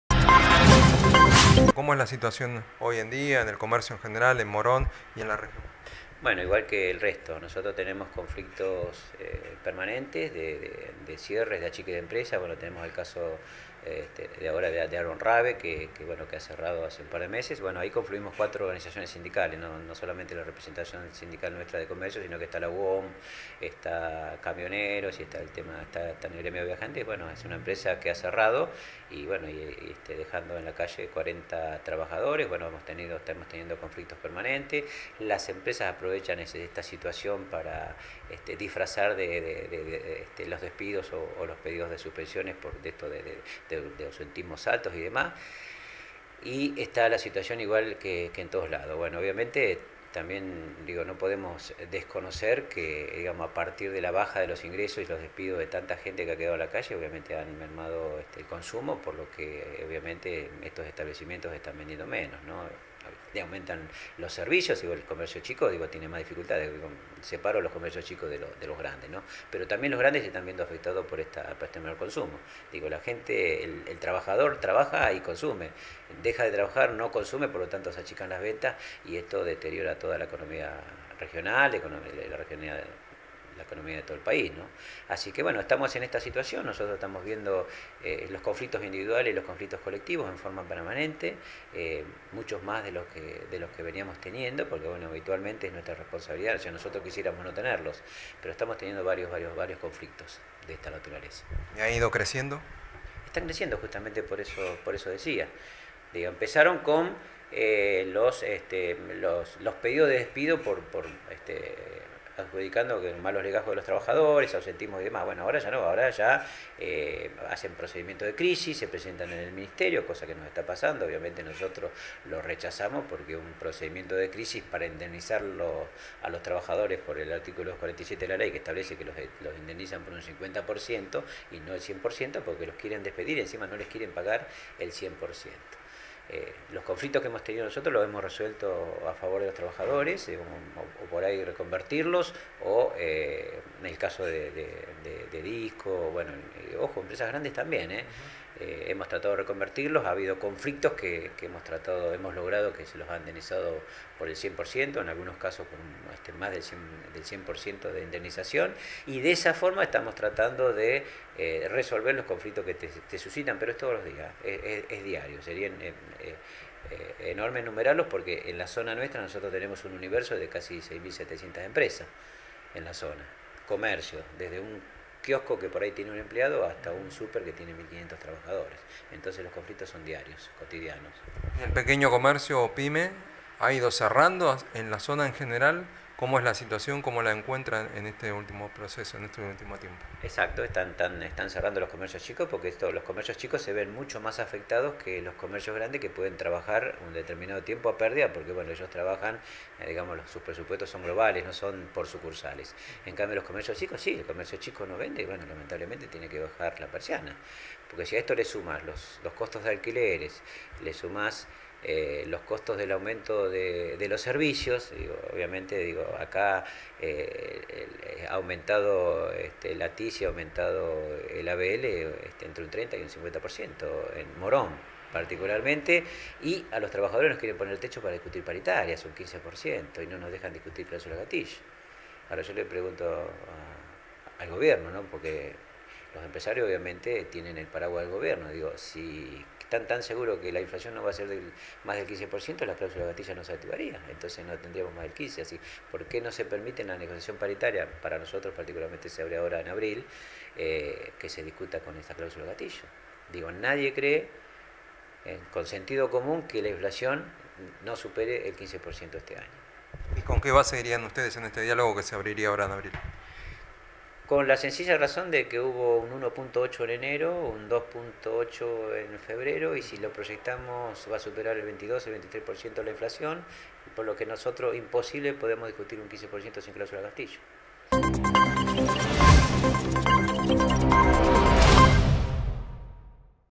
HUGO PINTOS - SEOCA MORON
El dirigente gremial de SEOCA Morón, se refirió a la situación del comercio local y la realidad que viven los empleados.